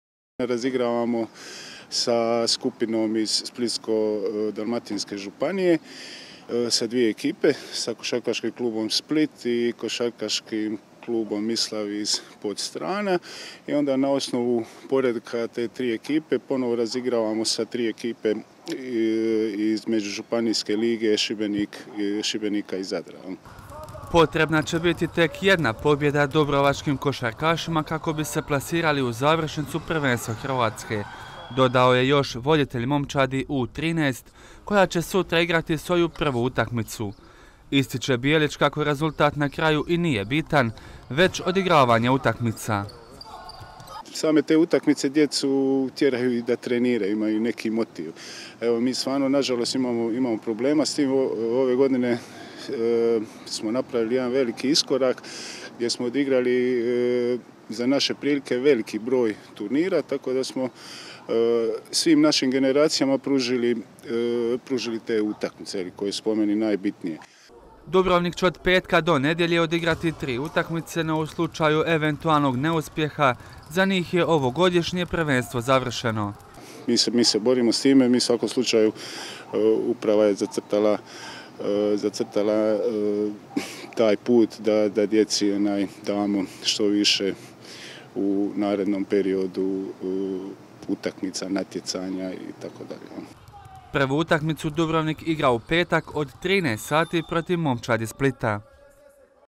KOSARKA - KK DUBROVNIK U-13 ZAPOČINJE PROTIV SPLITA NOVU SEZONU
ošarkaši Dubrovnika U-13 sutra će započeti svoje prvenstvo točnije završni turnir Regije jug. Iz kluba ističu kako visokih očekivanja nema, iako je za prolaz dalje potrebna jedna pobjeda!